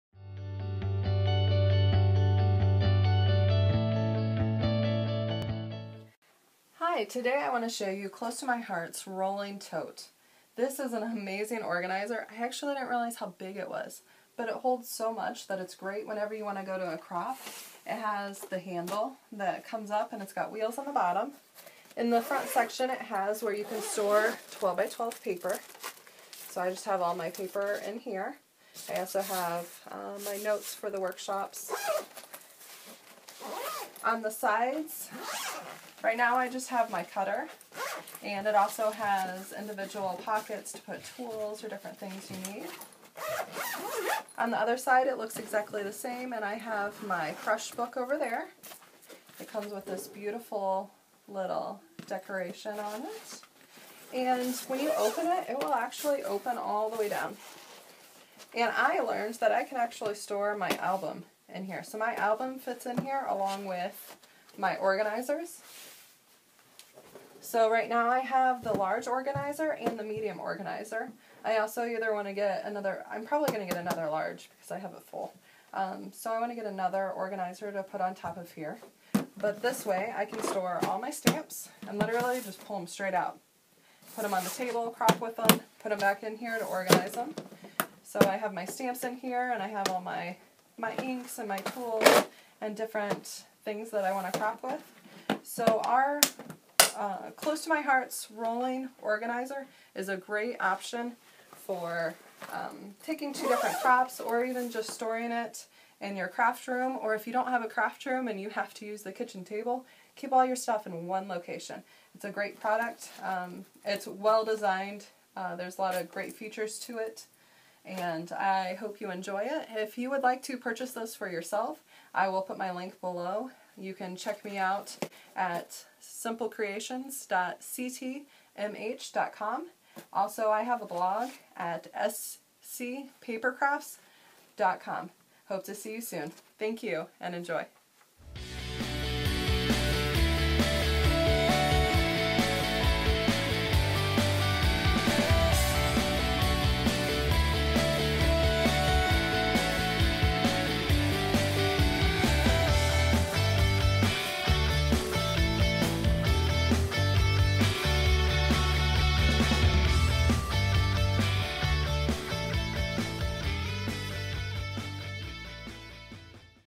Close to my Heart Rolling Tote
Need someway to organize all your scrapbooking supplies? Keep everything organized in the Close to my Heart Rolling Tote.  Let me so you how.